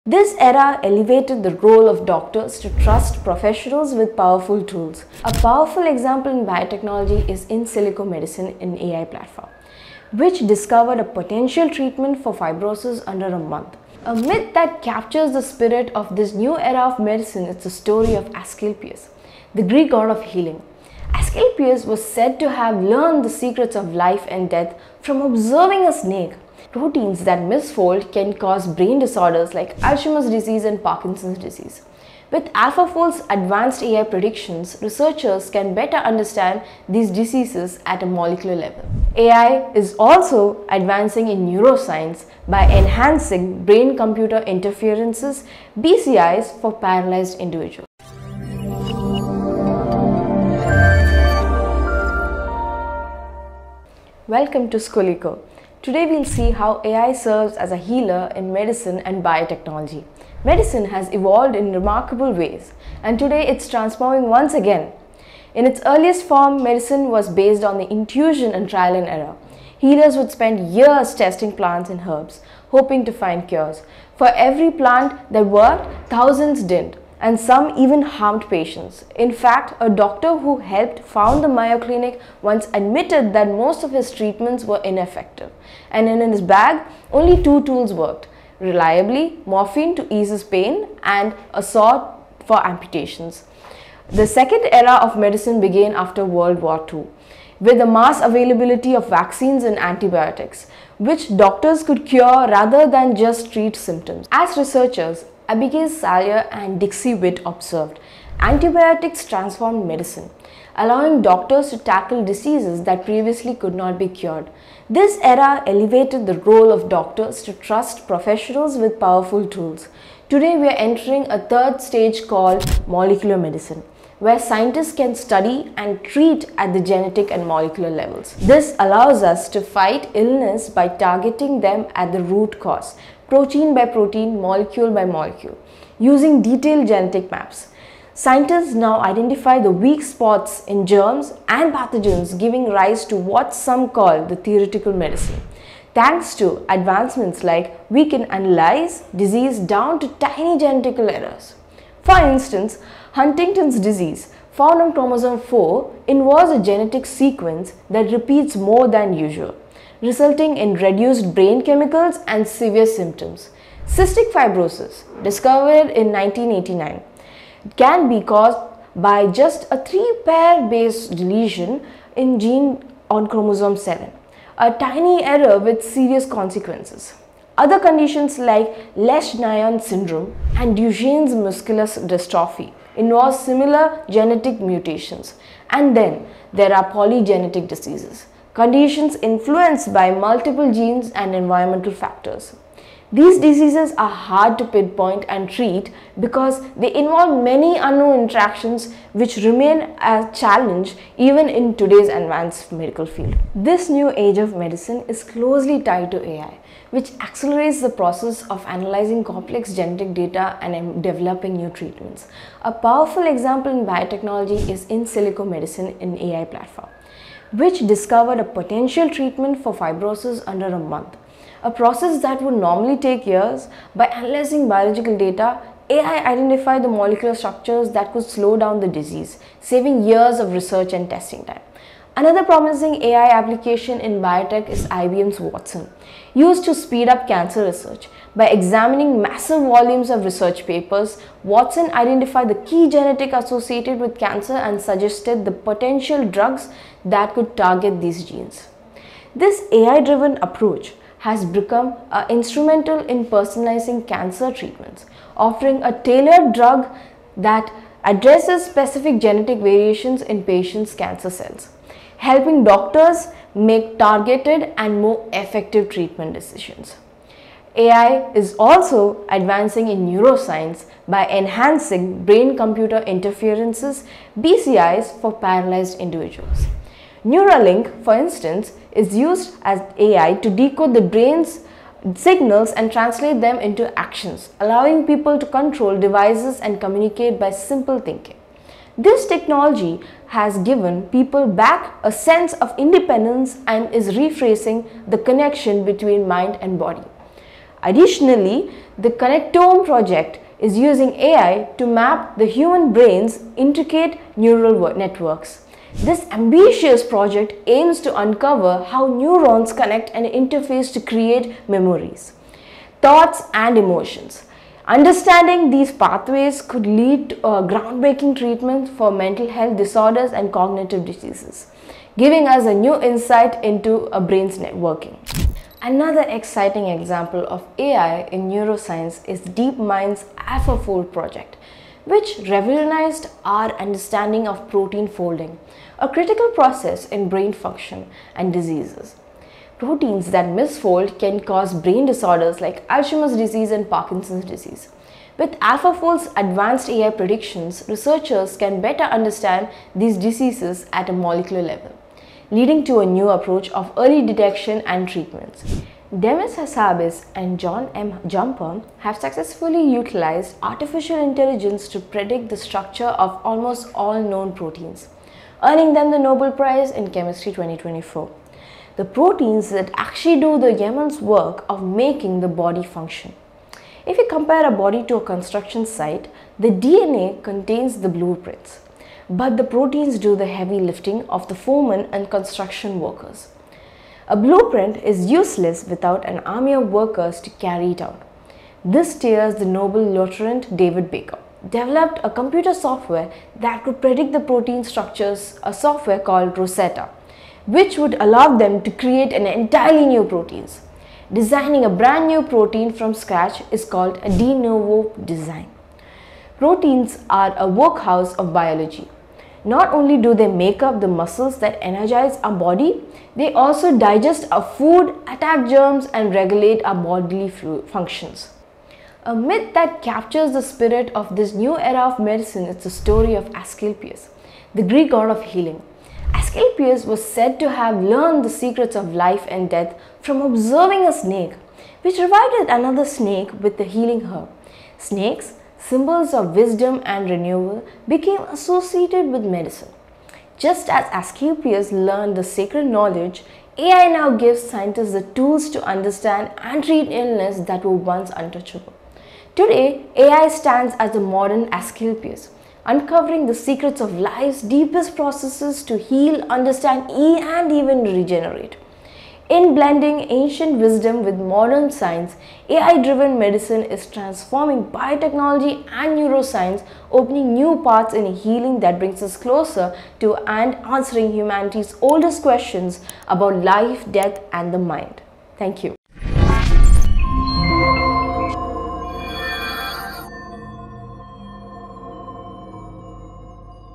The Lifesaving Alchemist: How AI is Transforming Biotechnology | Skoli Insights EP 01| Skoliko
Discover how Artificial Intelligence (AI) is revolutionizing biotechnology by acting as both a healer and medicine. From accelerating drug discovery and personalized treatments to early disease detection and regenerative medicine, AI is reshaping healthcare as we know it. Platforms like IBM Watson are personalizing cancer treatments, while AlphaFold is revolutionizing our understanding of protein folding, advancing neuroscience and medicine. Join us for an insightful journey into the powerful intersection of AI and biotechnology, where innovation meets healing!